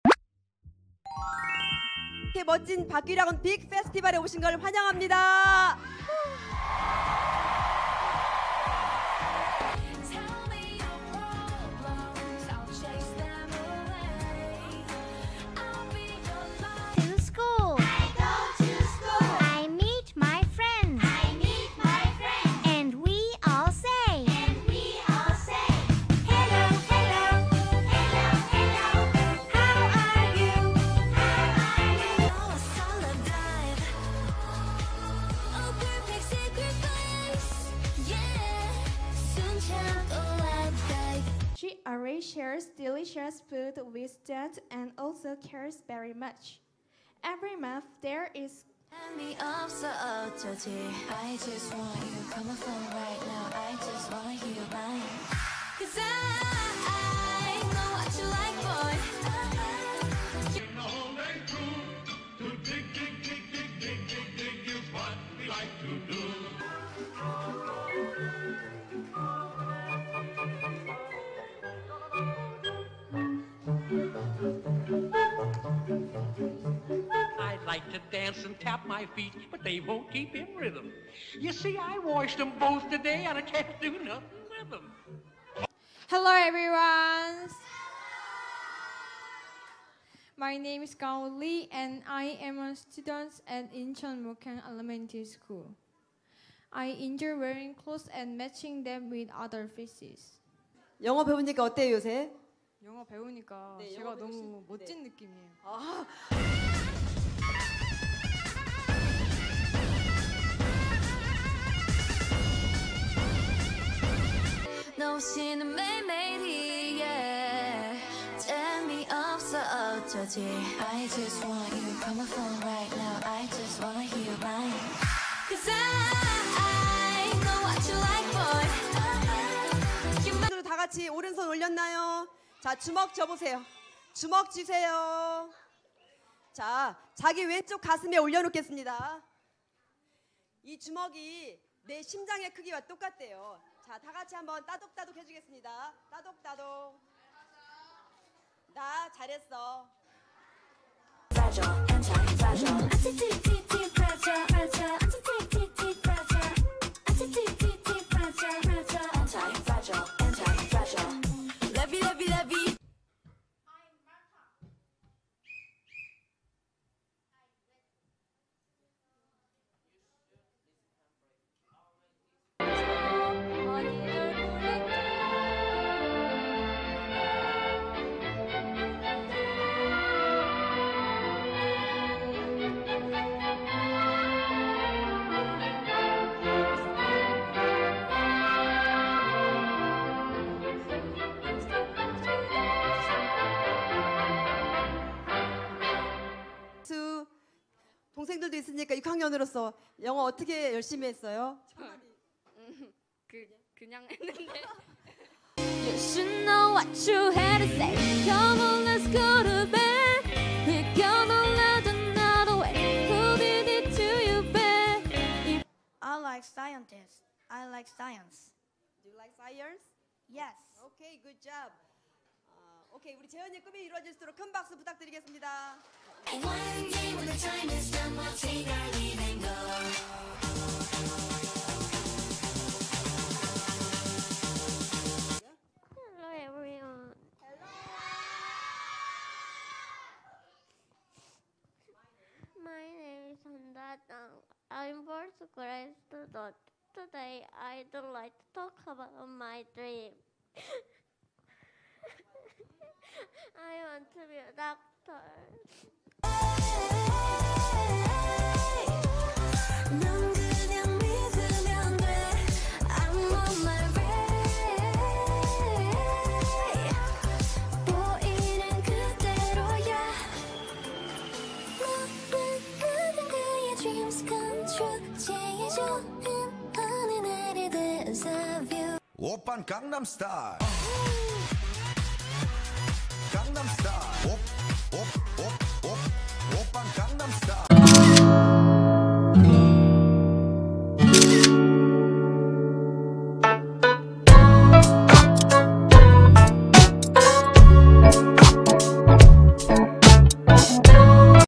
2023박규리학원빅페스티발 환영합니다 ♡❤️
초등학생때의 멋진 꿈을 펼치기위한 빅페스티발
오늘은 내가 내인생의 주연배우  뮤지컬.댄스.팝송.쏭.태권도등등 모두 신나고 행복합니다♡♡♡ 나의 꿈을 향한 멋진 행보는 시작되었다!!!